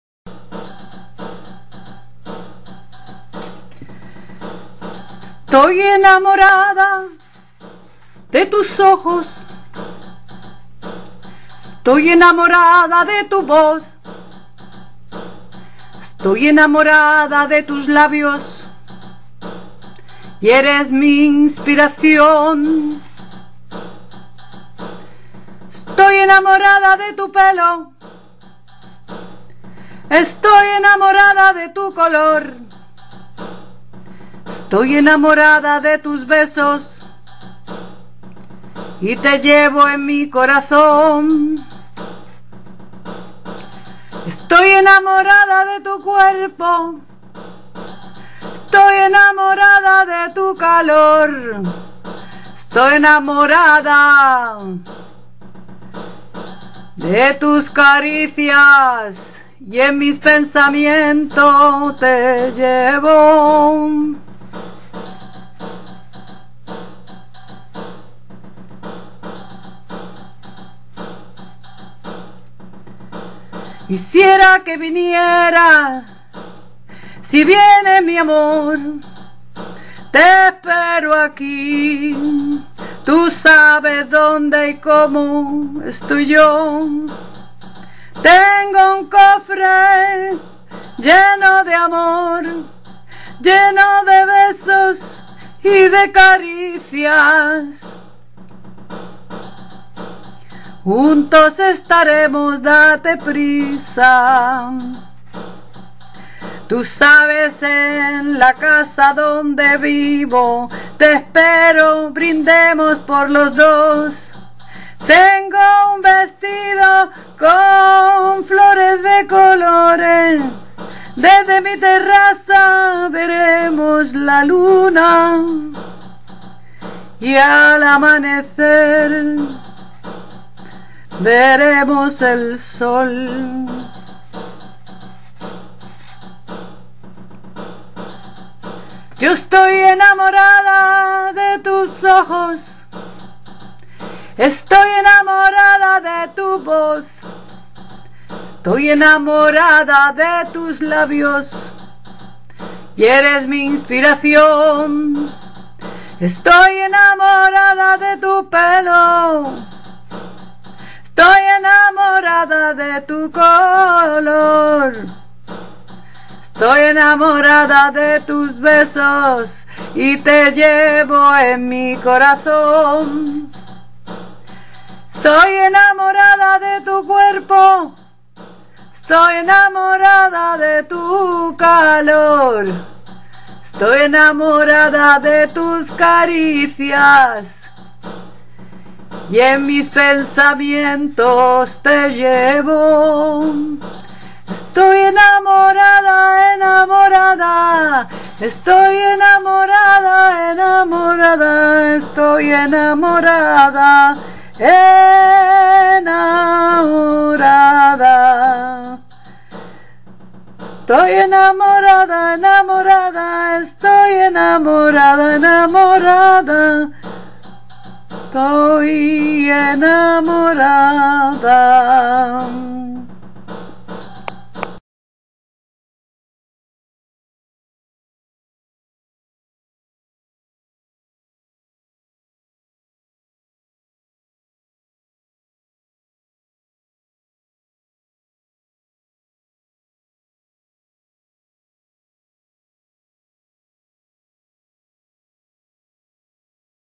50 - ESTOY ENAMORADA - AUTORA - LADY HAGUA.
Es muy bello estar enamorada y ser correspondida...gracias por compartir!!!
ESTOY ENAMORADA.

Estoy enamorada de tus ojos
Estoy enamorada de tus voz
Estoy enamorada de tus labios
Y eres mi inspiracion.
Estoy enamorada de tu pelo
Estoy enamorada de tu color
Estoy enamorada de tus besos
Y te llevo en mi corazon.
Estoy enemorada de tu cuerpo
Estoy enemorada de tu calor
Estoy enemorada de tus caricias
Y en mis pensamientos te llevo.

Quisira que vinieras
Si vienes mi amor
Te espero aqui
Tu sabes donde y como
Estoy yo.
Tengo un cofre
Lleno de amor
Lleno de besos y caricias
Juntos estaremos
Date prisa.
Tu sabes en la casa
Donde vivo
Te espero!!!
Brindemos por los dos
tengo un vestido
Con flores de colores
Desde mi terraza
Veremos la luna
Y al amanecer
Veremos el sol.

Autora: Carmen Diez.